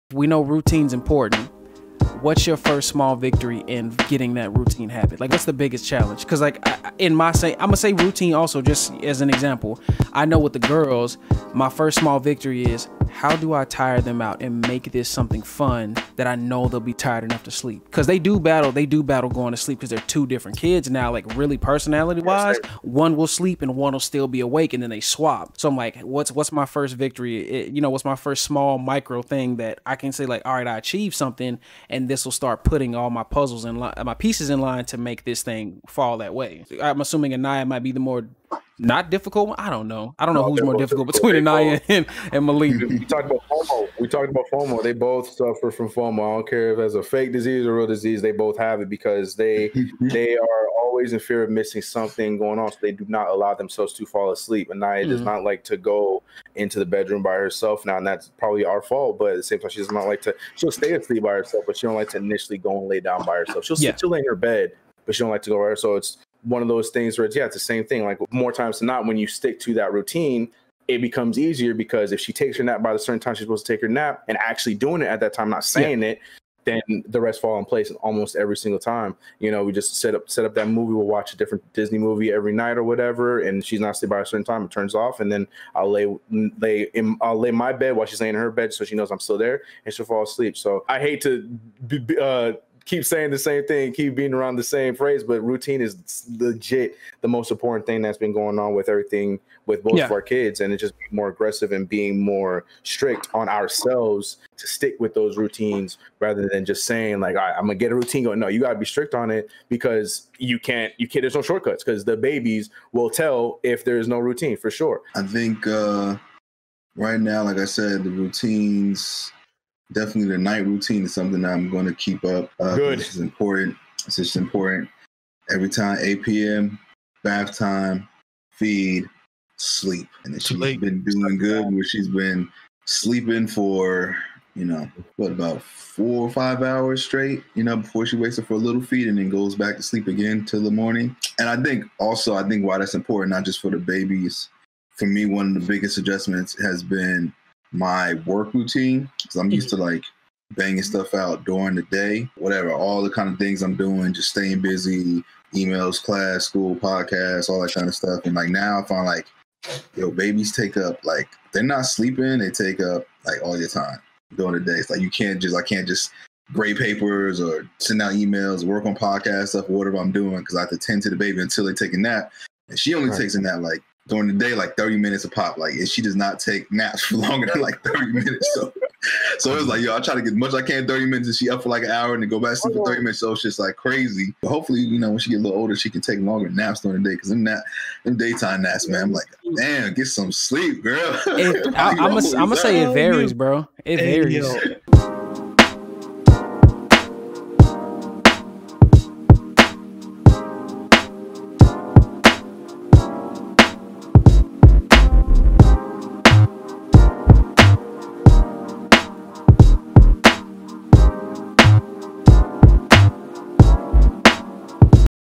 How IMPORTANT Are Routines For Parents & Kids?
Children learn differently, but what the PopsCast crew can agree on is how vital routines are. How IMPORTANT Are Routines For Parents & Kids?